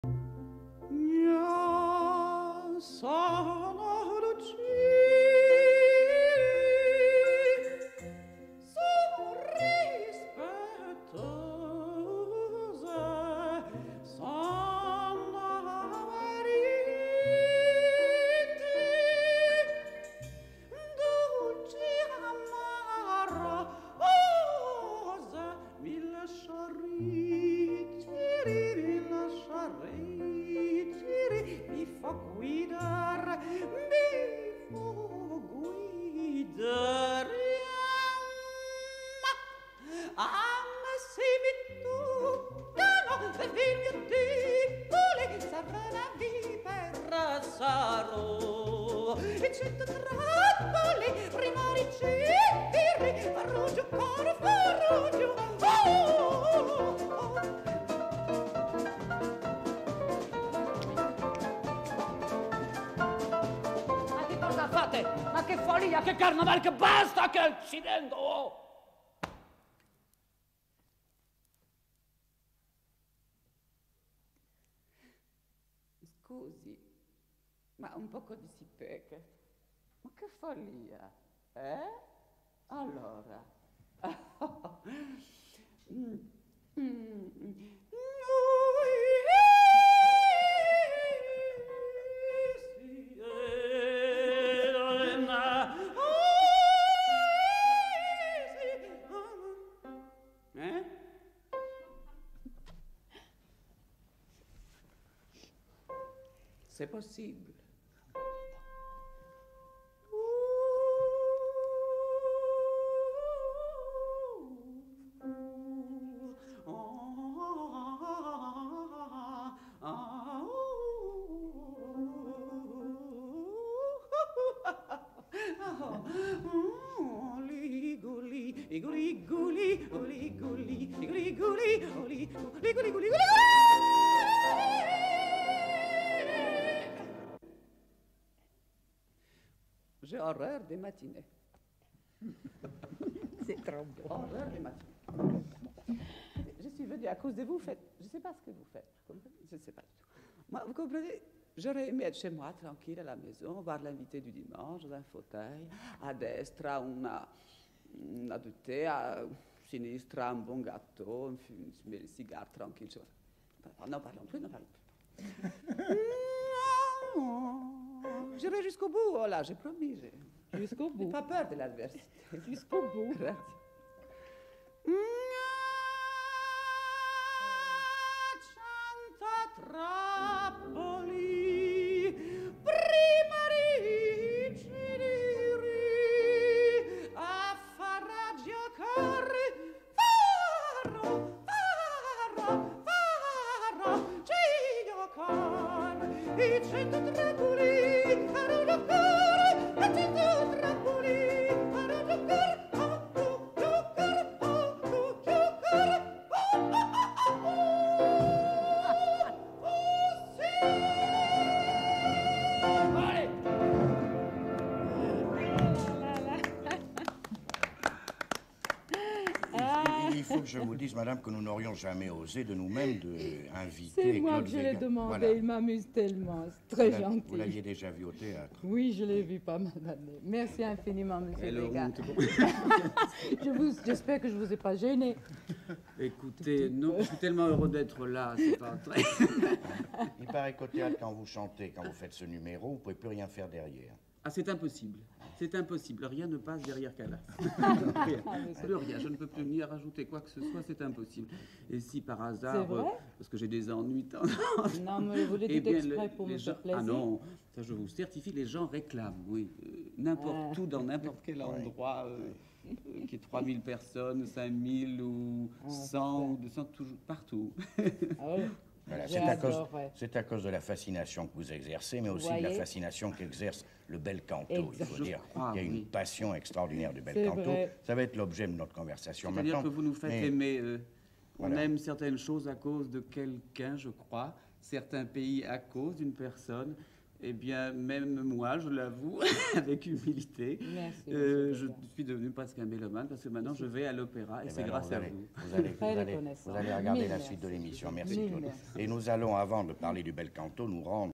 Claude Vega impersonating Maria Callas (20.04.1969)
Claude VEGA imitates Maria CALLAS, in her presence on the set. The diva laughs heartily at this outrageous imitation and then exchanges a few words with the artist.